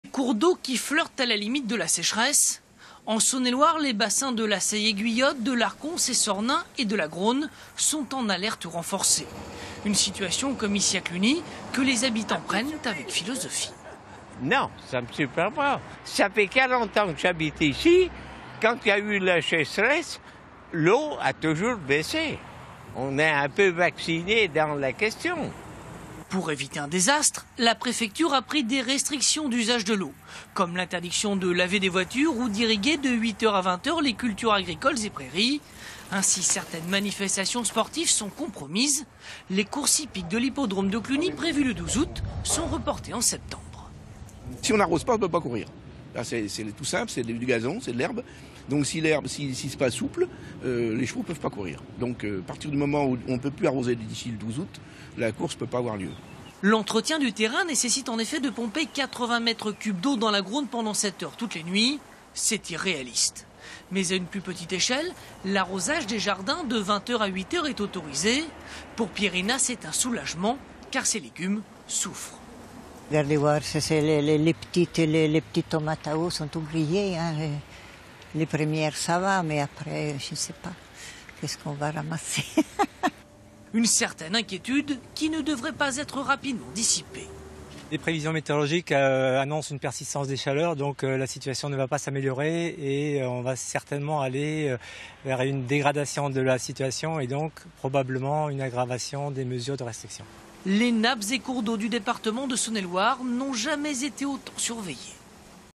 Canicule : la sécheresse gagne du terrain en Saône-et-Loire
En Saône-et-Loire, la principale conséquence de la canicule, c'est la sécheresse. Le niveau des cours d'eau commence à baisser. Comme dans l'Yonne et la Côte-d'Or précédemment, la préfecture vient de prendre des mesures de restrictions d'eau.

Reportage : Damien Boutillet, Lola Bodin-Adriaco, Vincent Chapuis, Aurélien Marone-Missud